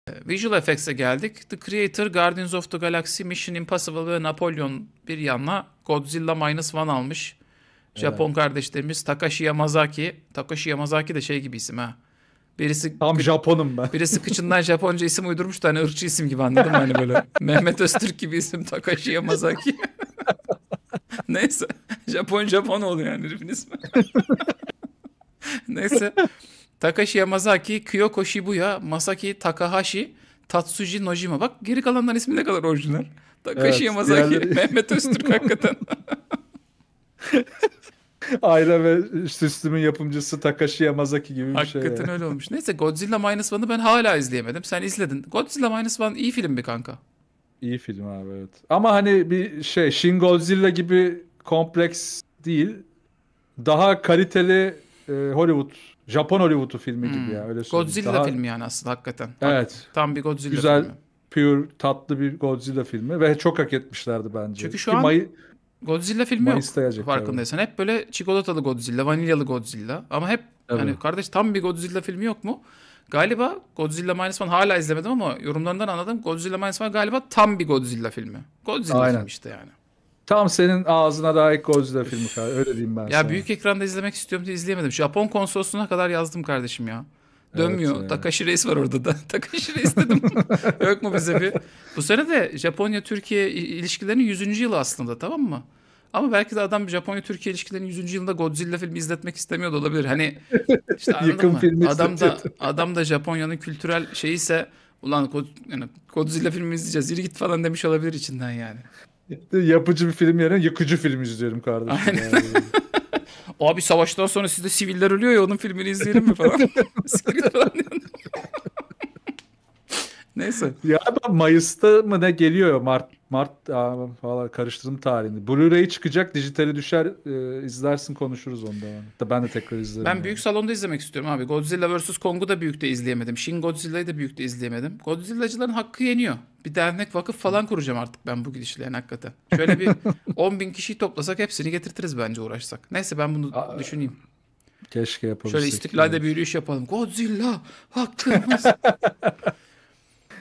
Oscar'ı kucağına oturtan Godzilla Minus One tam bir Godzilla filmi mi yoksa yarım mı?
Oscar'da en iyi görsel efekt ödülünü alan Godzilla'yı ülkemizde de büyük perdede izlemek için Can Sungur yürüyüş düzenlemek istiyor katılmaz mıyız?